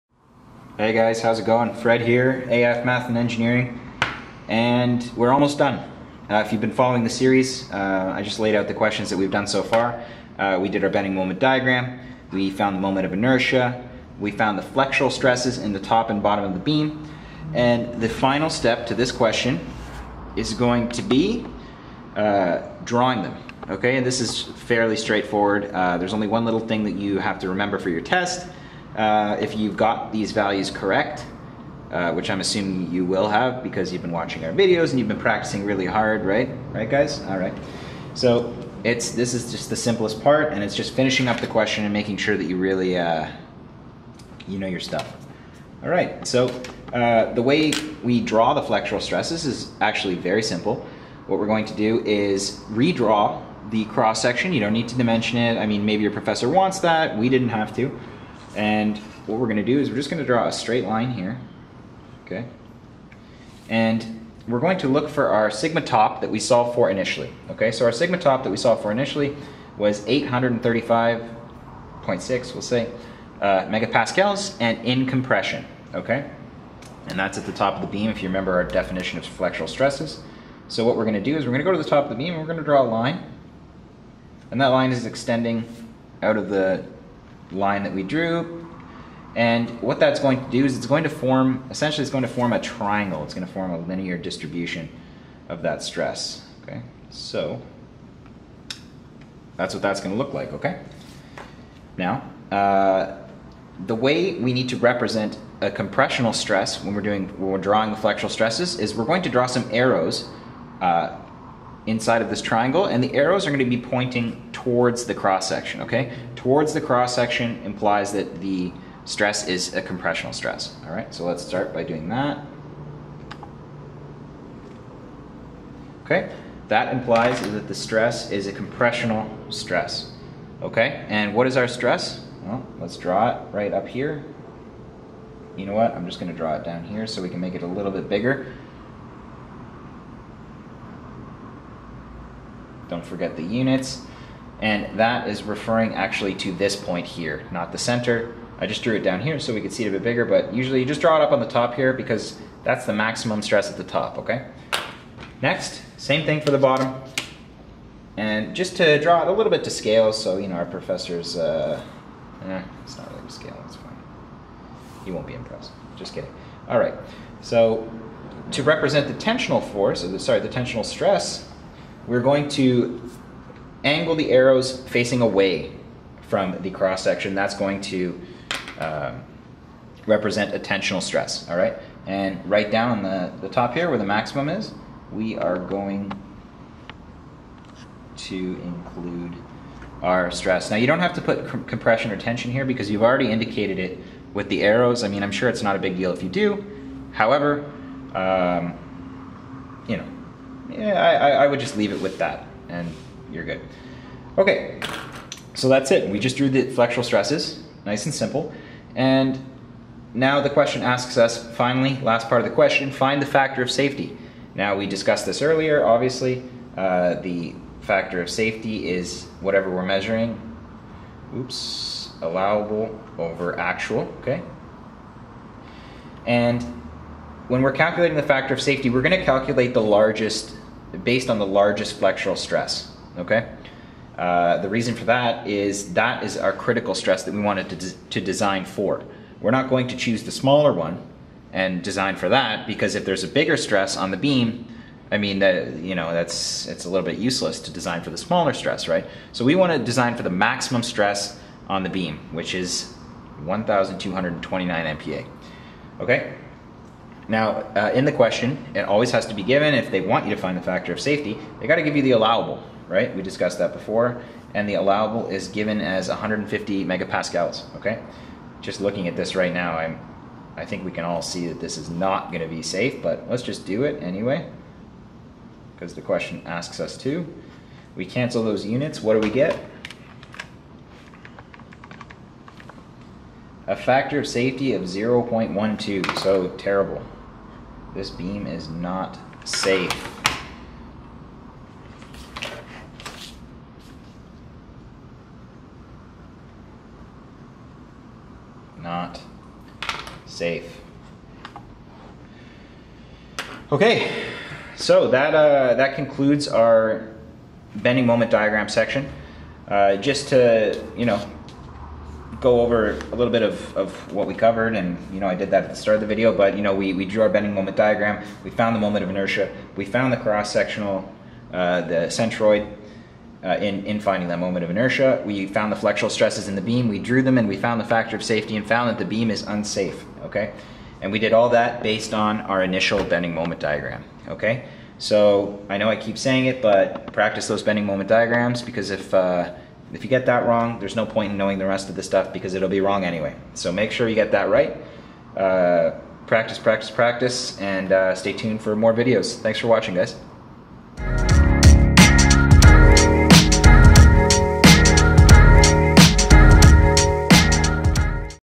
How to Calculate Flexural Stresses 2/2 - Bending Moment Diagram Part 7
In this video, we go over how to calculate the flexural stresses of the I-beam we've been working with for the past two videos.  An important topic in strengths of materials!

TimeStamp !

0:45 - Theory Ends - Solution Beings
1:00 - First Step - Start Drawing the "Top part of the I Beam"
2:35 - Second Step - Start Drawing the "Bottom part of the I Beam"
3:30 - Third Step - Find the "Factor of Safety"
4:43 - Final Solution - NOT SAFE! (Now try to do it on your own!!)

Note! This is a very LONG Question! Here are all the links you need: